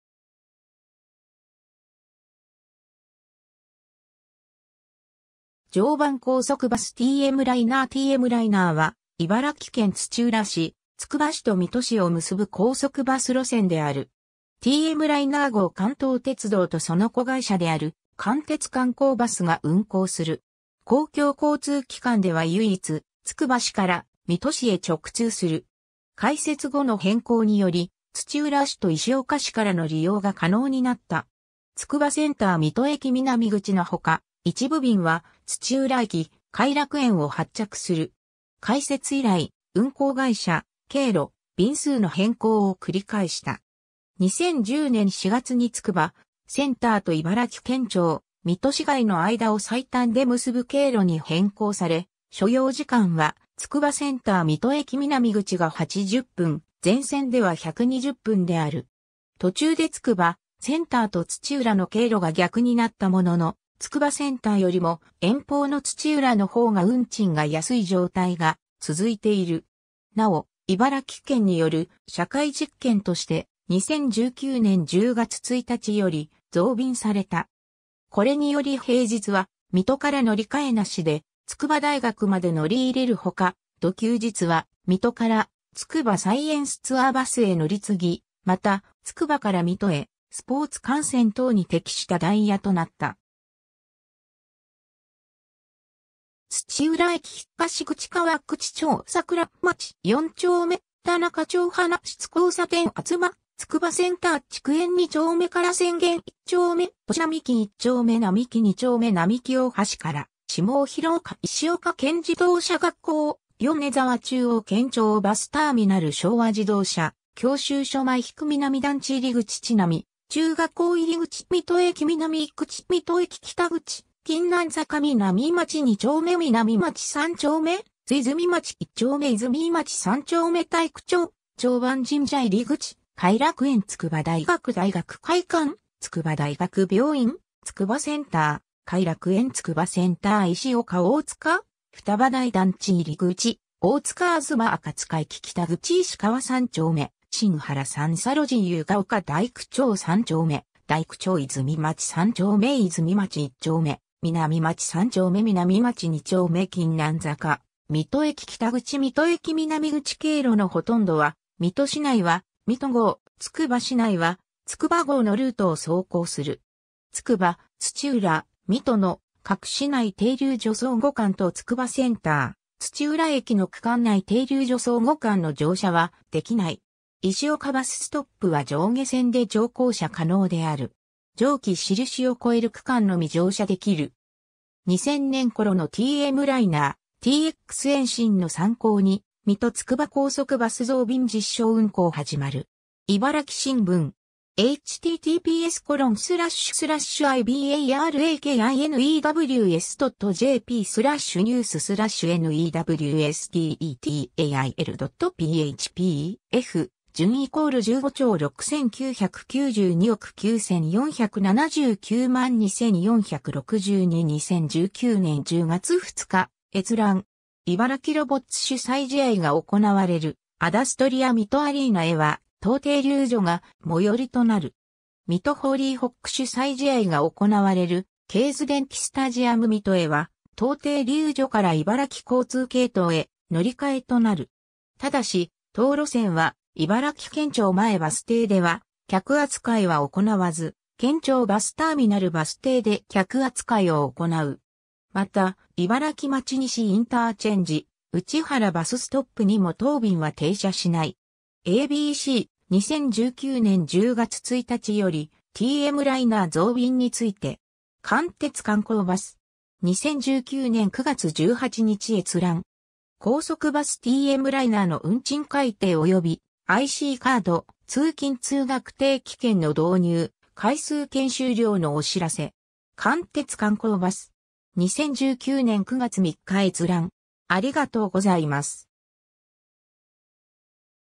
TMライナー
常磐高速バス 」 TMライナー TMライナー（ティーエムライナー）は、茨城県土浦市・つくば市と水戸市を結ぶ高速バス路線である。
 TMライナー号 関東鉄道（水戸営業所・土浦営業所）とその子会社である関鉄観光バスが運行する。
公共交通機関では唯一、つくば市から水戸市へ直通する。
開設後の変更により、土浦市と石岡市からの利用が可能になった。
つくばセンター - 水戸駅南口のほか、一部便は土浦駅・偕楽園を発着する。
 開設以来、運行会社・経路・便数の変更を繰り返した。
2010年4月につくばセンター（つくば駅）と茨城県庁・水戸市街の間を最短で結ぶ経路に変更され、所要時間はつくばセンター - 水戸駅南口が80分、全線（土浦駅東口～偕楽園）では120分である。
途中でつくばセンターと土浦の経路が逆になったものの、つくばセンターよりも遠方の土浦の方が運賃が安い状態が続いている。
 なお茨城県による社会実験として、2019年10月1日より増便された。
これにより平日は水戸から乗り換え無しで筑波大学まで乗り入れる他、土休日は水戸からつくばサイエンスツアーバスへ乗り継ぎ、また、つくばから水戸へスポーツ観戦等に適したダイヤとなった。
つくば・土浦・水戸の各市内停留所相互間とつくばセンター - 土浦駅の区間内停留所相互間の乗車はできない。
石岡バスストップは上下線で乗降車可能である。
上記★印を越える区間のみ乗車できる。
2000年頃のTMライナー（水戸駅にて） ^ “TX延伸の参考に　水戸-つくば　高速バス　増便実証運行始まる”. 茨城新聞. (2019年10月2日). 2019年10月2日閲覧。
 ^ 茨城ロボッツ主催試合が行われるアダストリアみとアリーナへは当停留所が最寄りとなる。
 ^ 水戸ホーリーホック主催試合が行われるケーズデンキスタジアム水戸へは、当停留所から茨城交通系統へ乗換となる。
 ^ ただし、当路線は「茨城県庁前」バス停では客扱いは行わず、「県庁バスターミナル」バス停で客扱いを行う。
また、「茨城町西IC」、「内原バスストップ」にも当便は停車しない。
 ^ a b c “2019年10月1日(火)より「TMライナー(水戸～つくば)」増便について”. 関鉄観光バス (2019年9月11日). 2019年9月18日閲覧。
 ^ “高速バスＴＭライナーの運賃改定及びICカード・通勤通学定期券の導入、回数券終了のお知らせ”. 関鉄観光バス (2019年9月3日). 2019年9月3日閲覧。